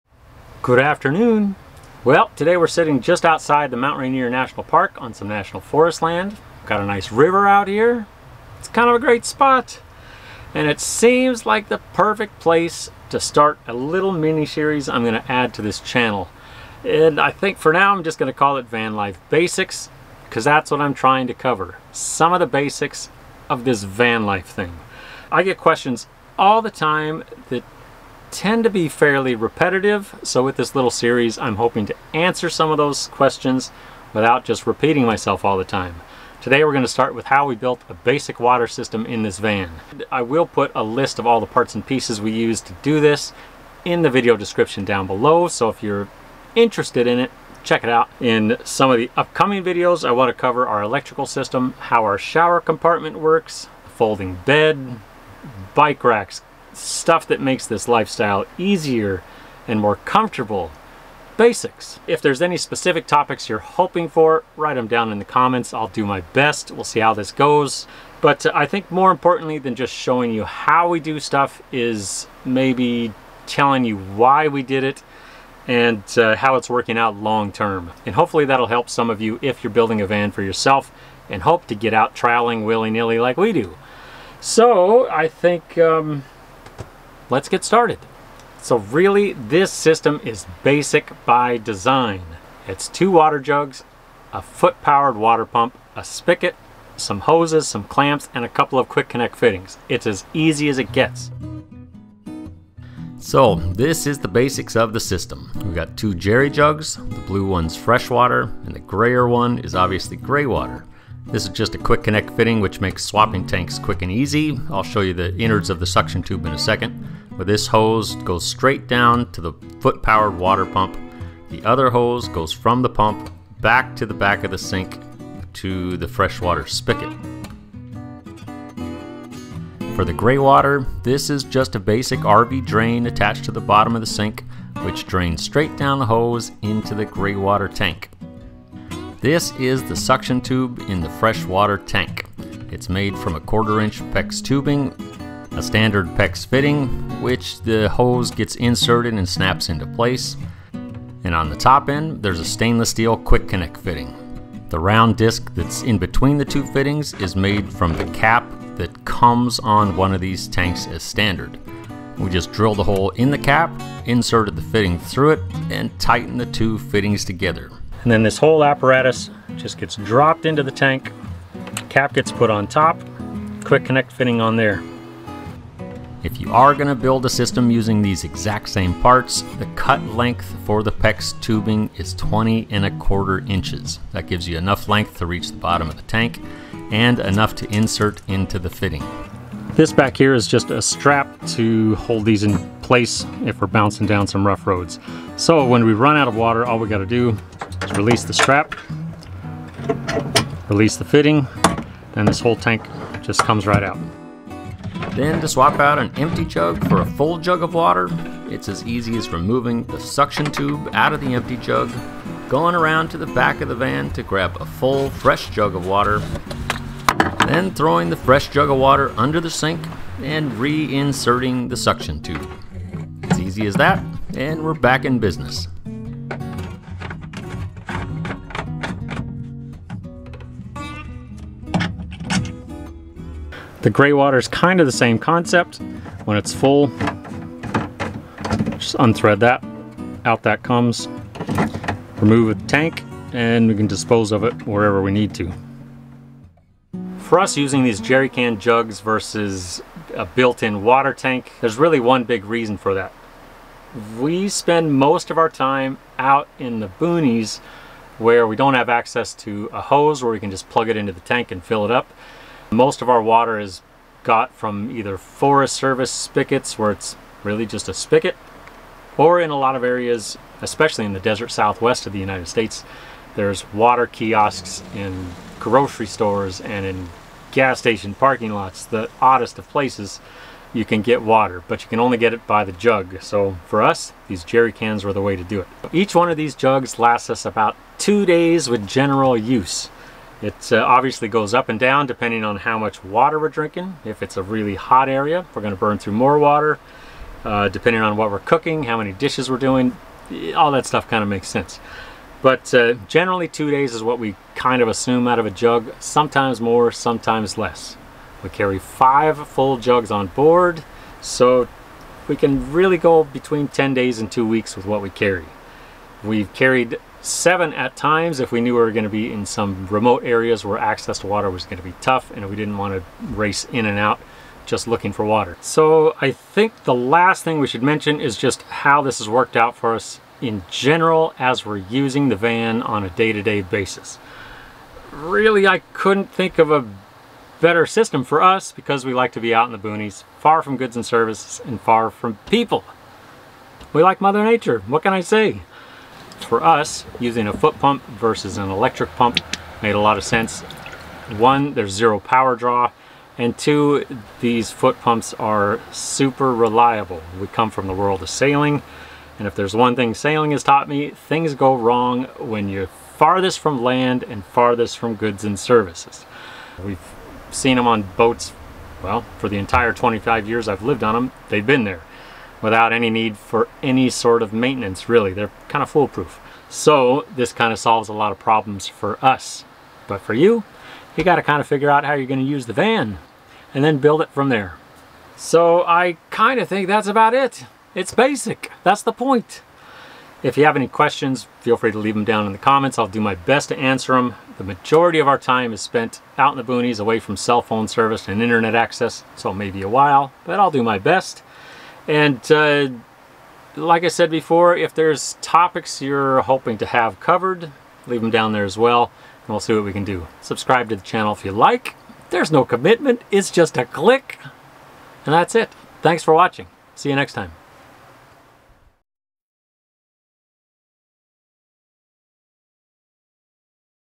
How to Build a Basic Water System For Your Van - Vanlife Basics - Episode 1
This is the start of a new mini-series I am adding to this channel called Vanlife Basics, and in it, we're going to cover some of the basics, starting with how to build a basic water system for your van.

The Parts List for the Basic Water System:

6 Gallon Blue Fresh Water Jug

6 Gallon Grey Water Jug

Stainless Steel Quick Connect Fittings

Sink Drain/Strainer Assembly

Sink Drain Hose System

Hose Clamps

1/4" Pex Tubing: this was purchased at a local Home Depot as a 3' remnant

Pex Quick Connect Fitting